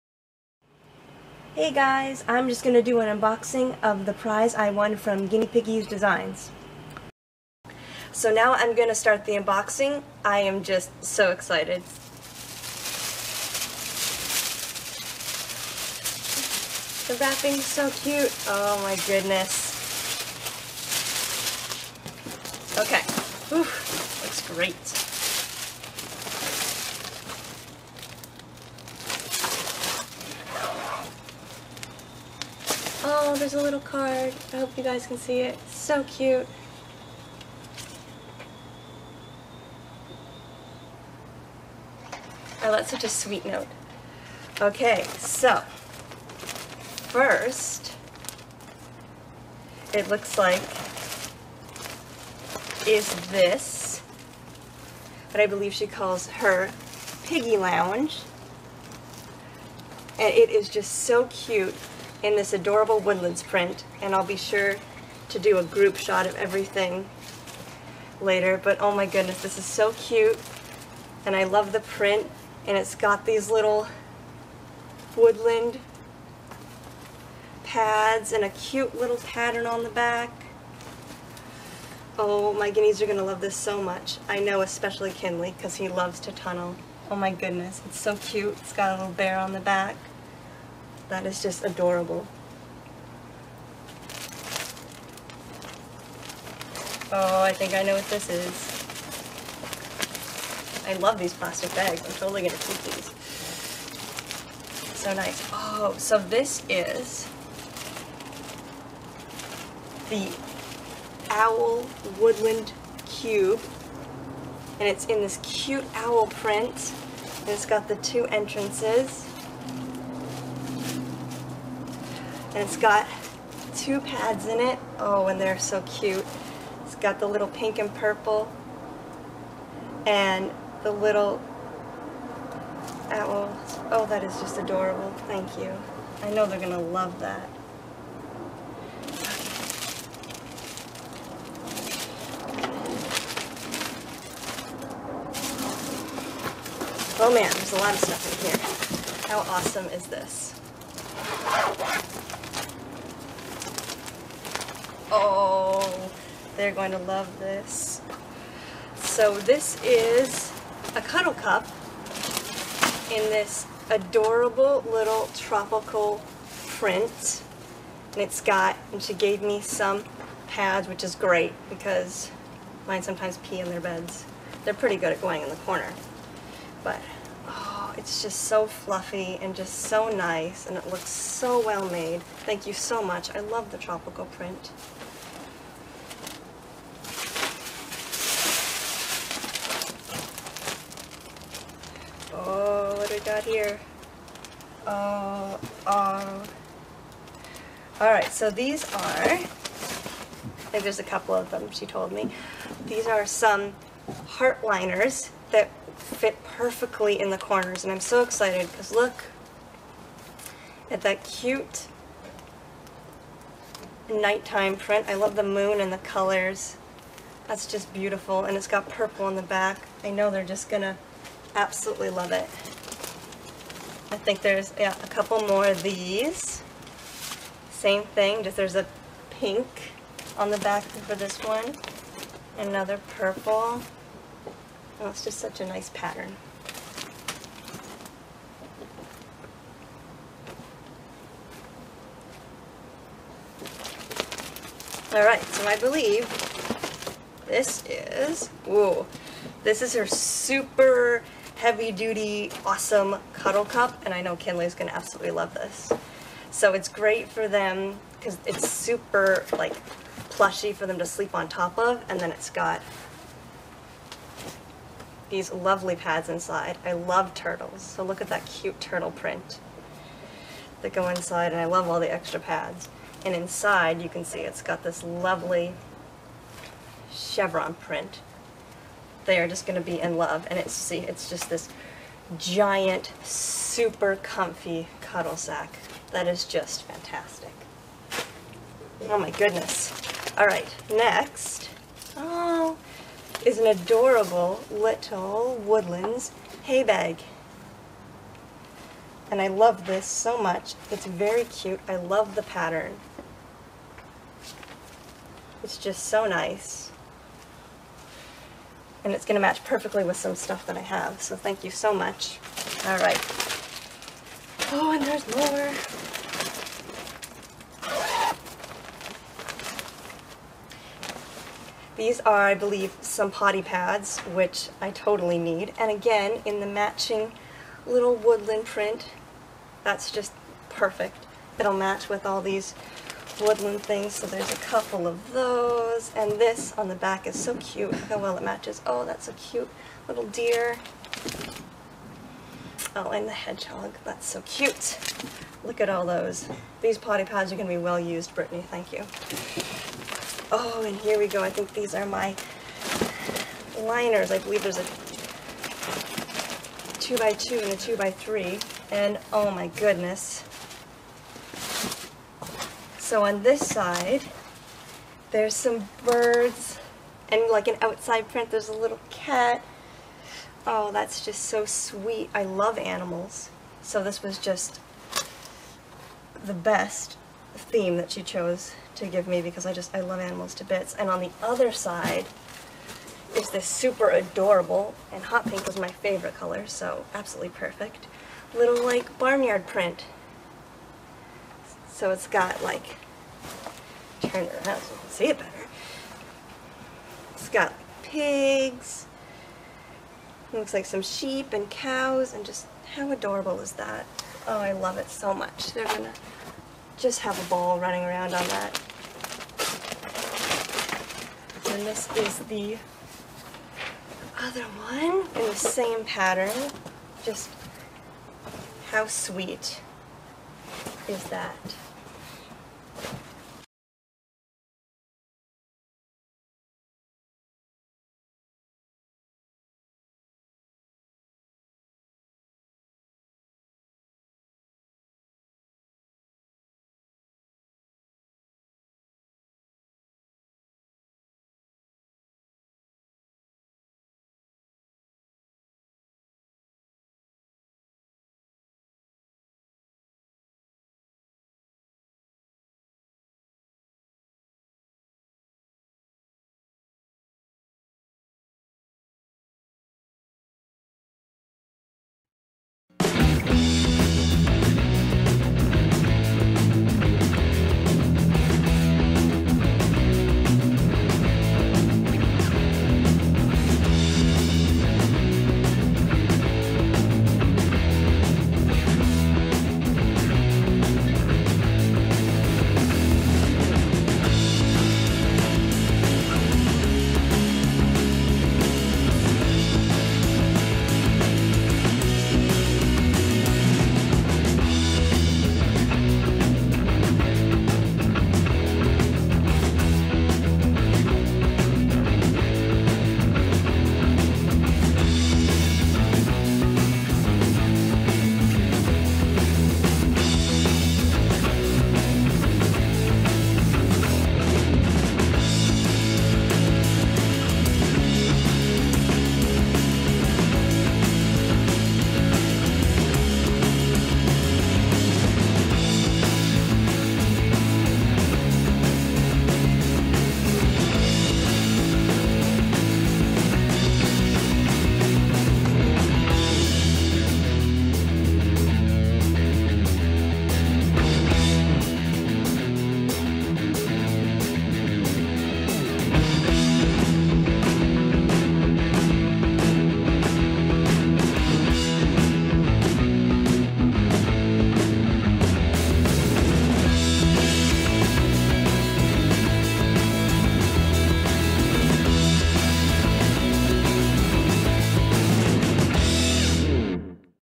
I won Guinea Piggies Designs 50 Subscriber Giveaway!
Thank you so much to Guinea Piggies Designs. I love everything so much. I really appreciate all the hard work and effort you put into my prize. My guinea pigs LOVE everything.

If you would like to check out Brittany's awesome work. Some of her social media links are below!